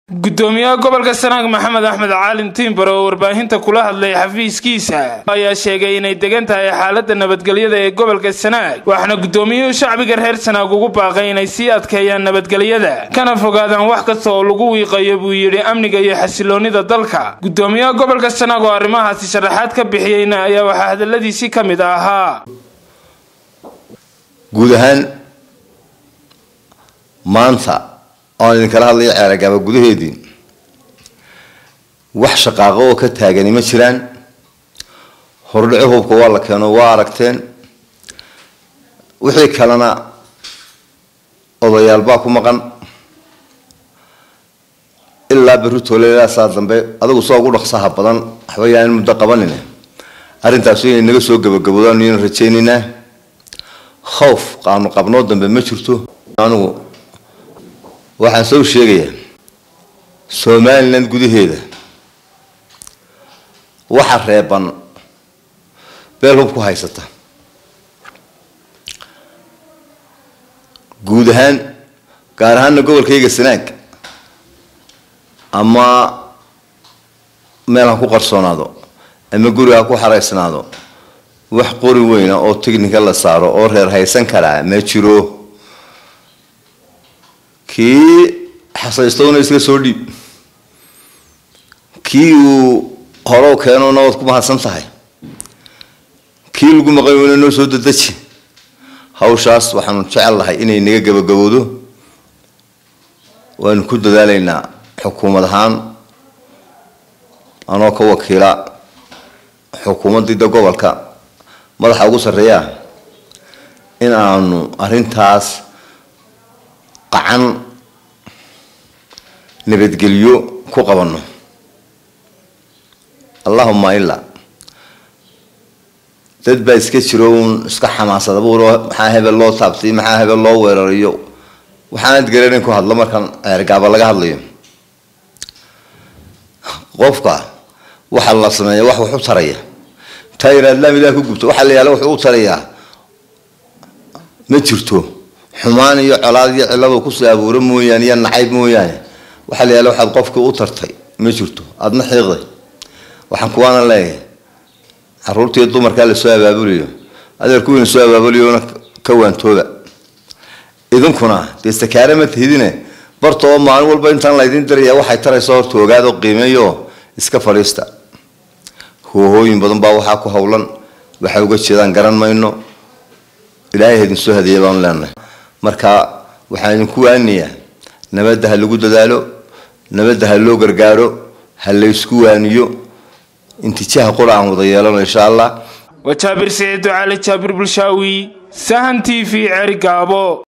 Deg Deg Daawo Badhasaabka Gobolka Sanaag Oo Jabhada Caare Ka Hadlay
★ ★ ★Geeska Afrika Channel★ ★ ★
Reported By Jaabir Siciid Ducaale(Jaabir Bulshaawi)Erigavo /Somaliland